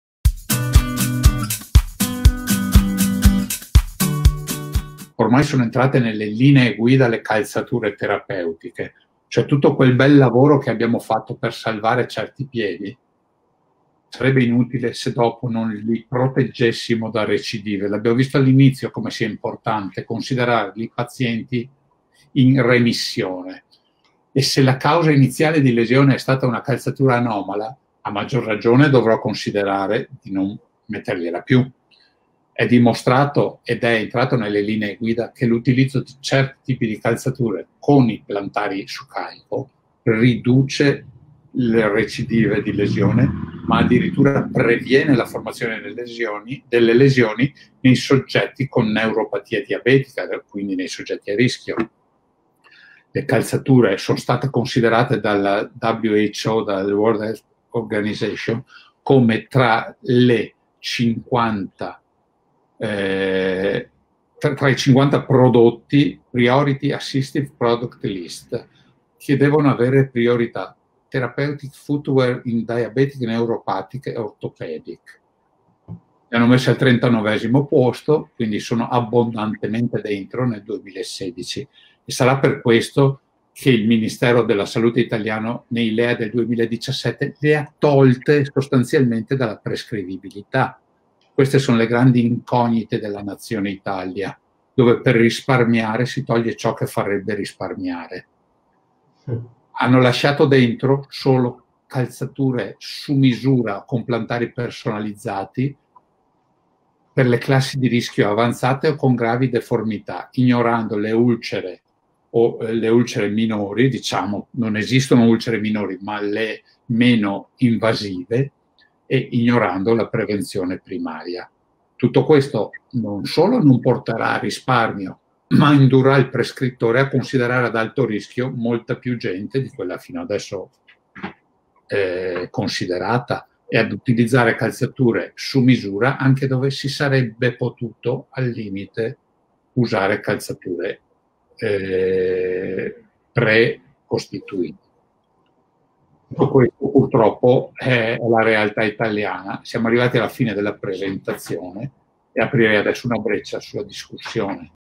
Le Calzature nella patologia del Piede diabetico.
Piede diabetico: quando scegliere la calzatura corretta per il proprio benessere e per prevenire l'ulcerazione in caso di neuropatia diabetica.

La complicanza più comune nel diabete è il piede diabetico con neuropatia e conseguente mancanza di sensibilità. Per i pazienti affetti da neuropatia diabetica e con ulcere diabetiche  è fondamentale anche fare  attenzione alle calzature che si indossano.

Linee guida e calzature terapeutiche: come prevenire le recidive. Dopo la guarigione è fondamentale evitare le ricadute e prevenire le lesioni.

01:27 I LEA del 2017

Per risparmiare si toglie ciò che farebbe risparmiare. Viene ignorata la prevenzione primaria.
L'utilizzo inadeguato di scarpe non adatte è una delle cause più diffuse delle ulcere diabetiche al piede. Indossare calzature sbagliate espone ad rischio i piedi,favorendo l’insorgenza del piede diabetico.
Tutto il lavoro svolto dallo specialista, per curare l'ulcera, svanisce se la calzatura non è adeguata.
Se la causa d'insorgenza dell'ulcerazione fosse dovuta ad una calzatura inadatta dovremmo provvedere alla sostituzione con una calzatura più performante.

Per informazioni:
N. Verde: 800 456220

Dove siamo
FocusMed S.r.l. - Largo Traiano, 4, 35036 Montegrotto Terme (PD)

Diabetic foot: when to choose the right foot wearing for our own wellness and to avoid the diabetic foot ulcers when the diabetic neuropathy is present.

The most frequent complication in the diabetic patients is the diabetic foot lesion, with concomitanti neuropathy and subsequent loss of sensibility. Patients affected by diabetic neuropathy and with diabetic ulcers have also to care to their foot wearings.

Guidelines and therapeutical footwears: how to avoid the relapses. After the healing is fundamental to avoid recurrences and to prevent the new lesions.

01:27 I LEA del 2017